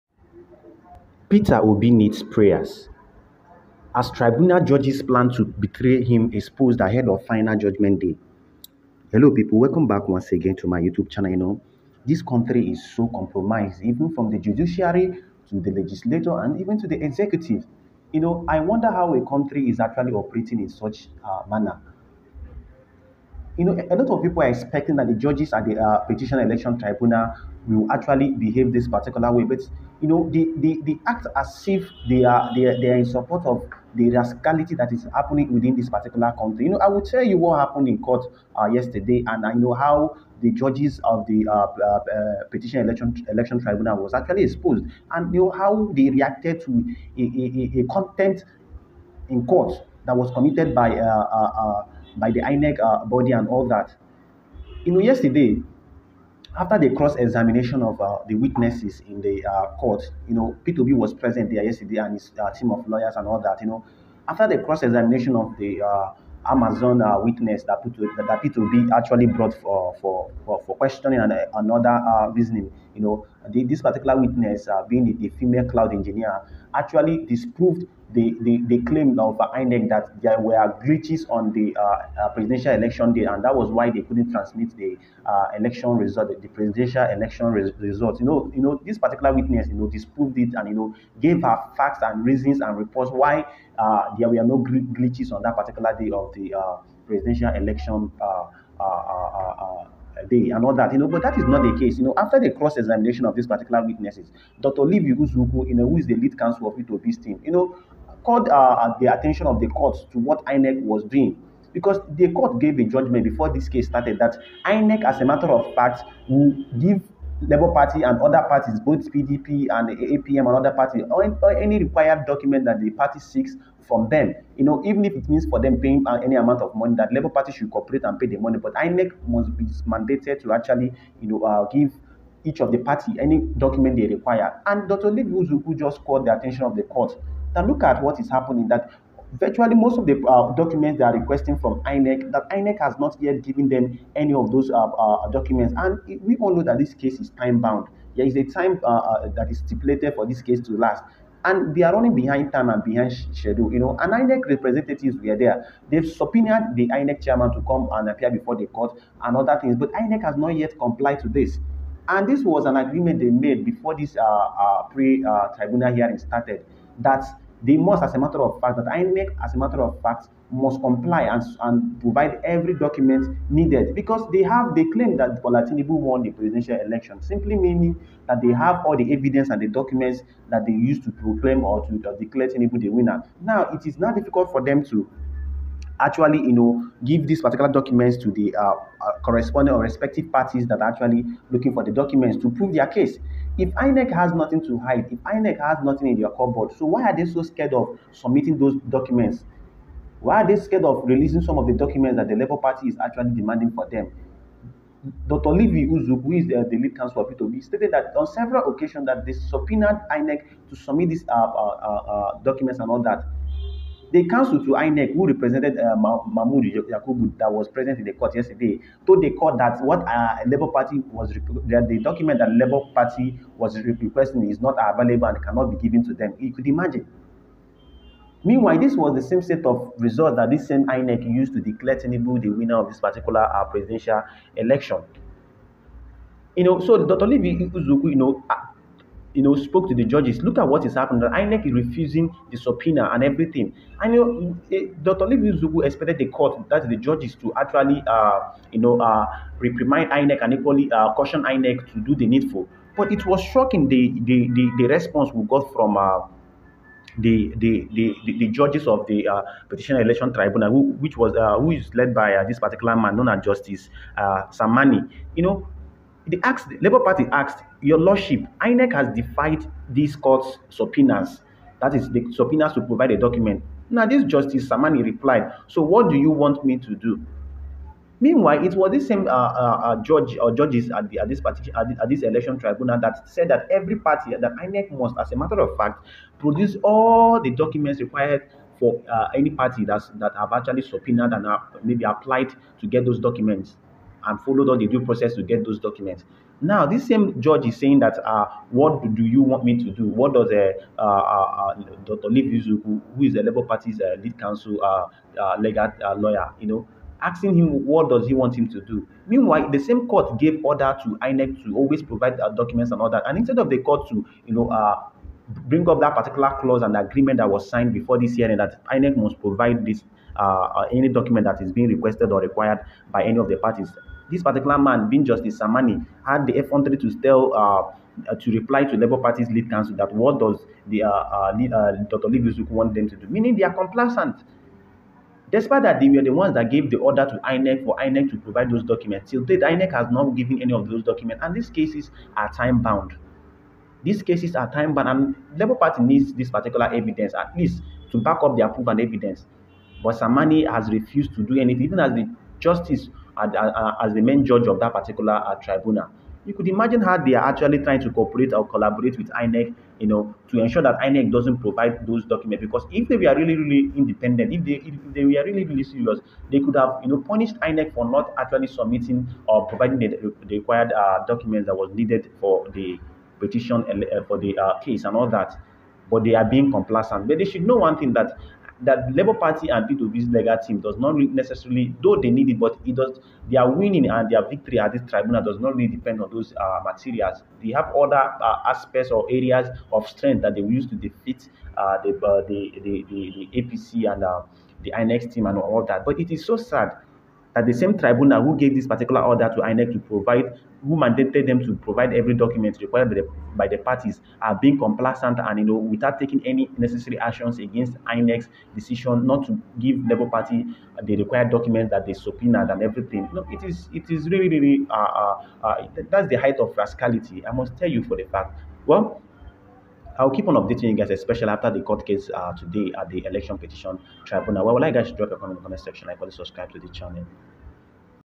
Peter Obi Need Prayers, Tribunal Judges Plan To Betrày Him £xp0sed Ahead Of Final Judgement
Peter Obi election petition Tribunal court live updates and information are being analyzed on this channel as well as Bola Tinubu eligibility and Inec credibility.